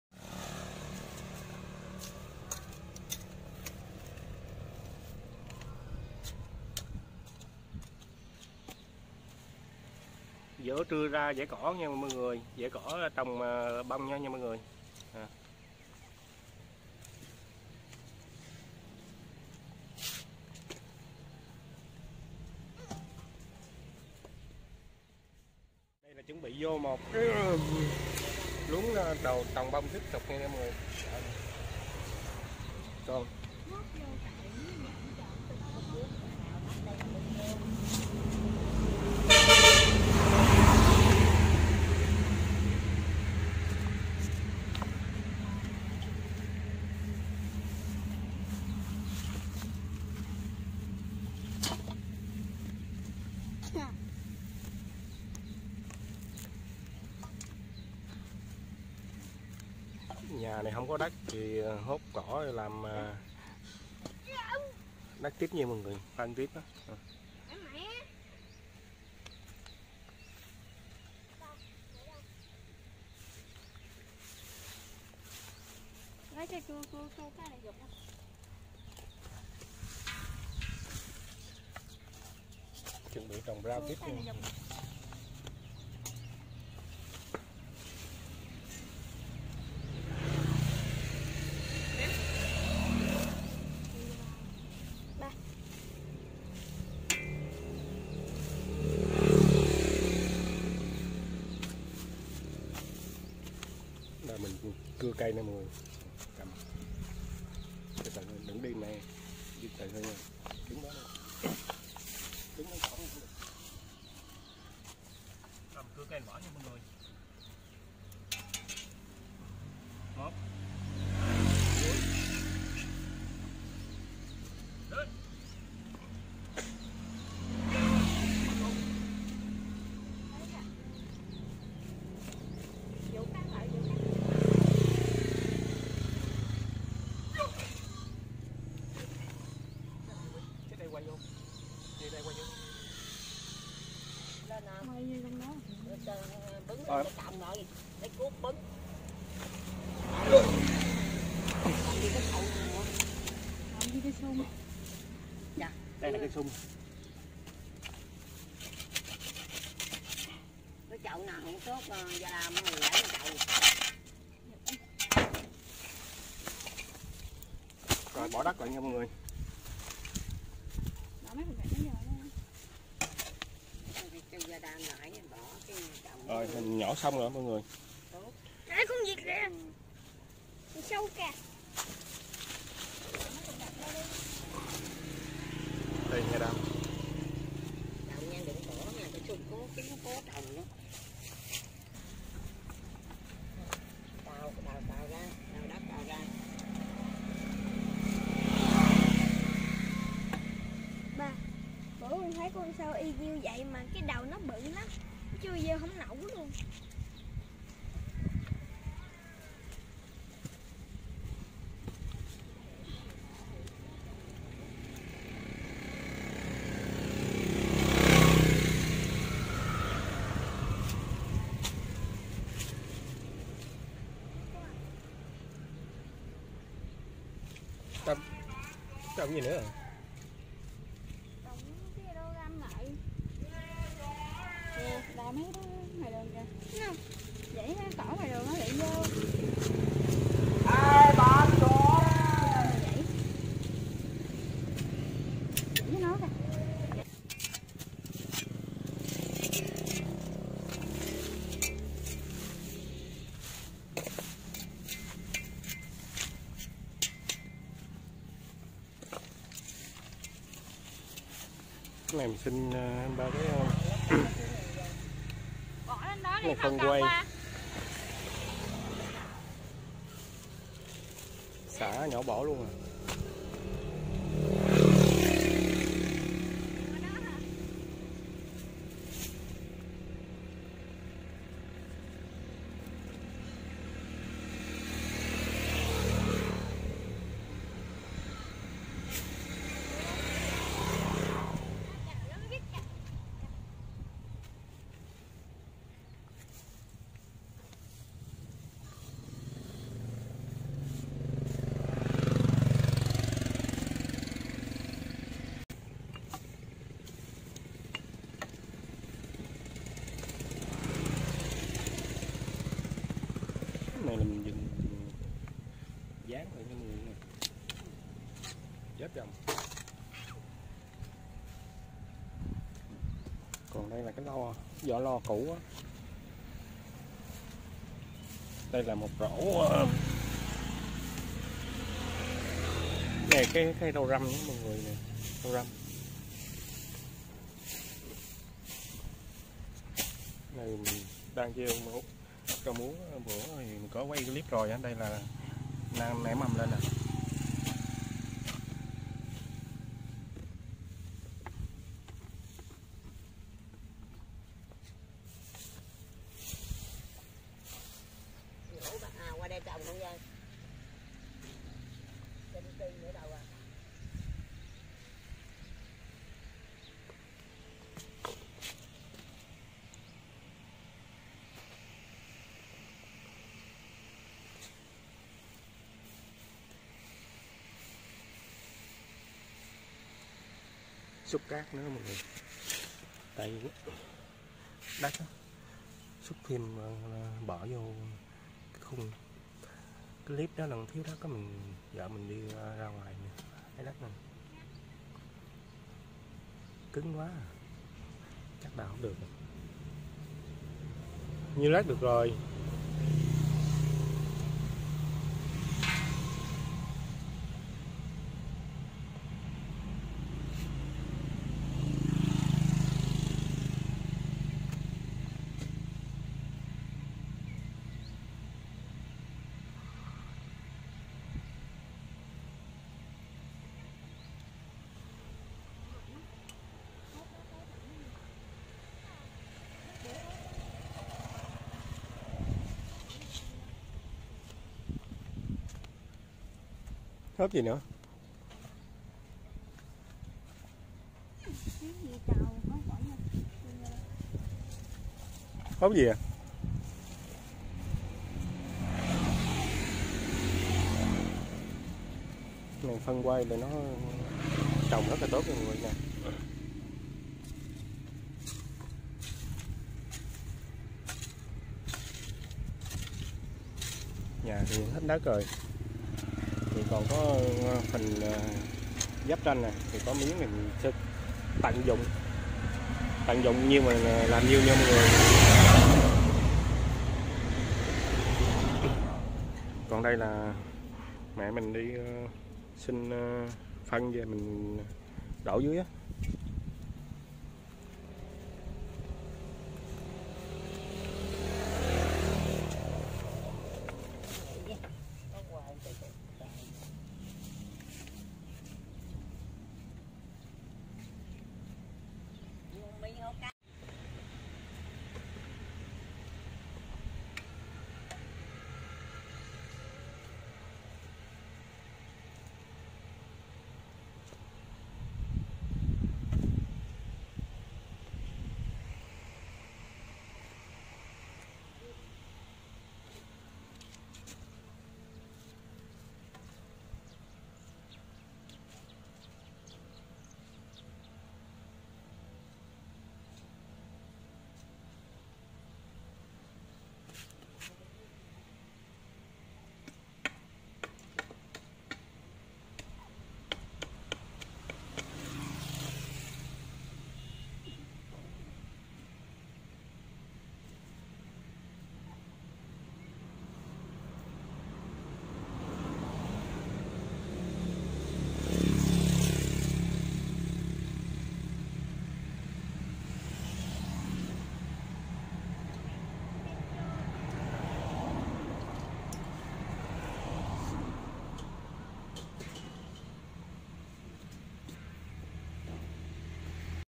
vợ chồng Minh, trưa nắng vợ với đứa con dọn dẹp cỏ cho vườn rau nhỏ
cảm ơn quý vị đã xem video giúp vợ chồng Minh